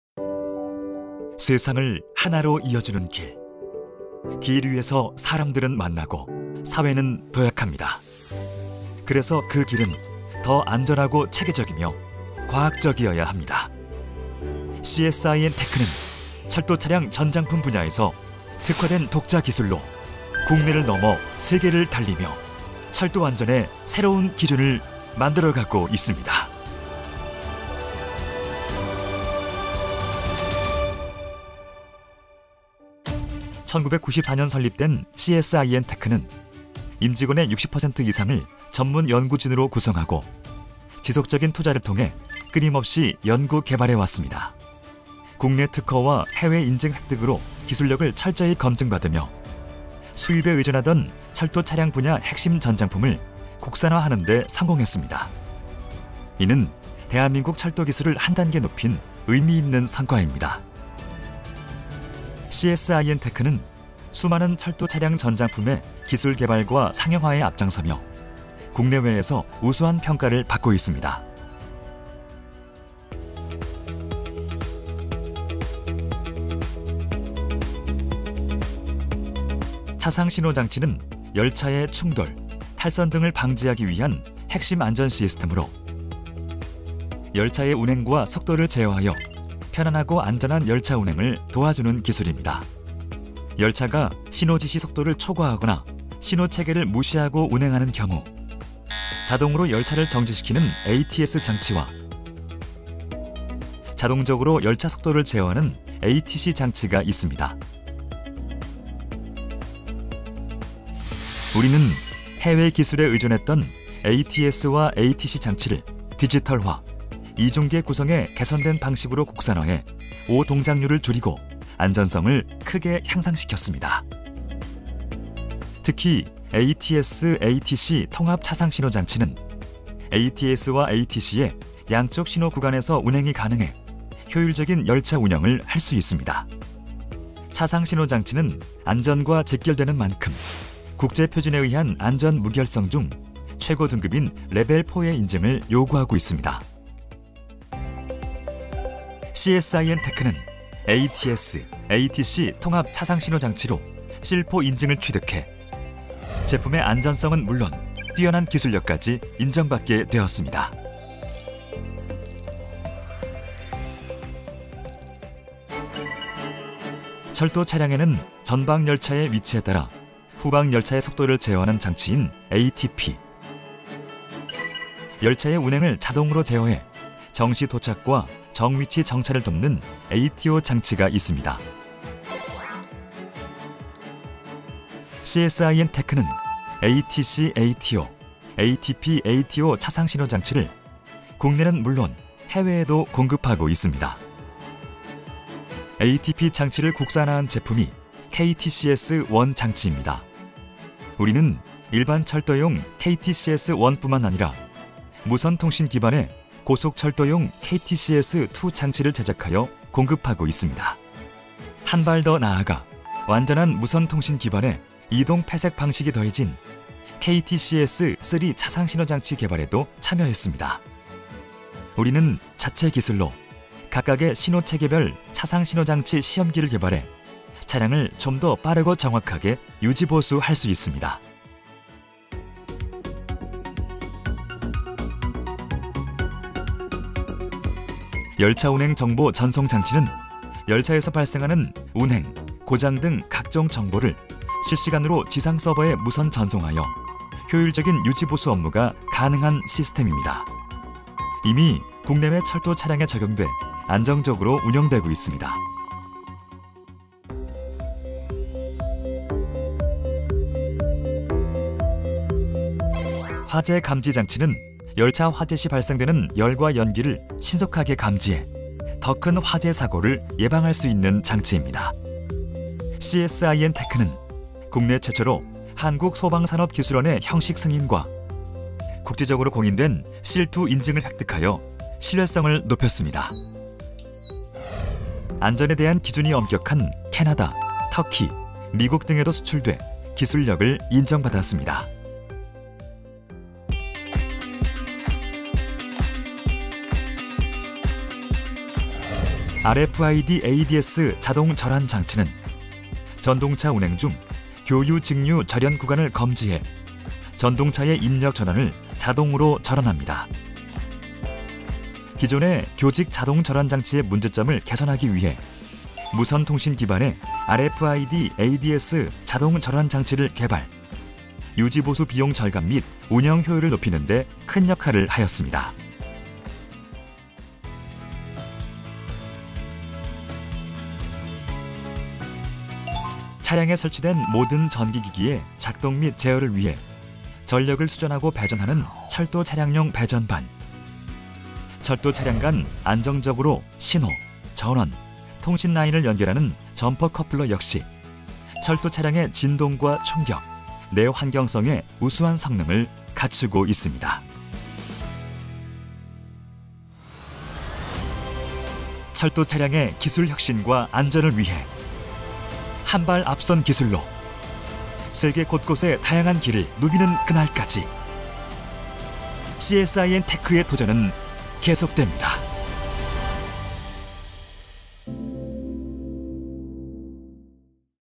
2023년 씨에스아이엔테크 회사소개 영상 (국문)
철도차량 전장품 전문기업 씨에스아이엔테크